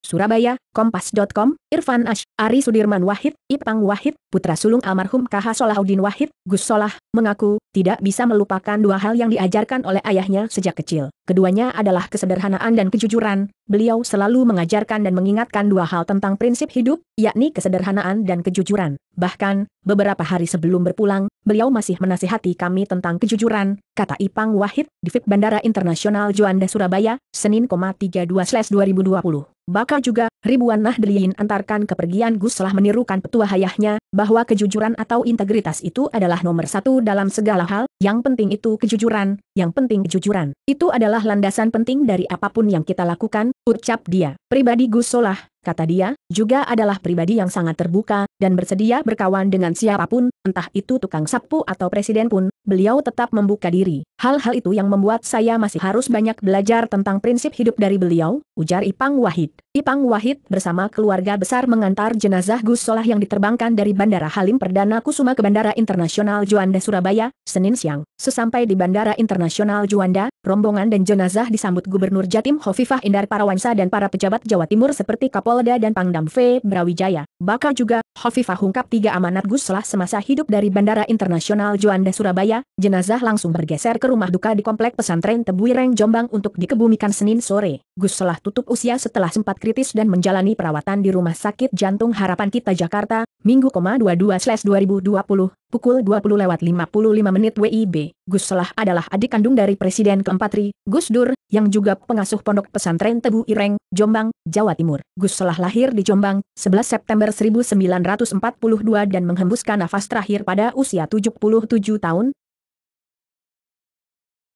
Putra Gus Sholah Tidak Bisa Lupakan 2 Nasihat Ayahnya Ini
SURABAYA, KOMPAS.com - Irfan Asy,ari Sudirman Wahid ,Ipang Wahid,, putra sulung Almarhum KH Sholahudin Wahid ,Gus Sholah, mengaku, tidak bisa melupakan 2 hal yang diajarkan oleh ayahnya sejak kecil. Keduanya adalah kesederhanaan dan kejujuran. ,Beliau selalu mengajarkan dan mengingatkan 2 hal tentan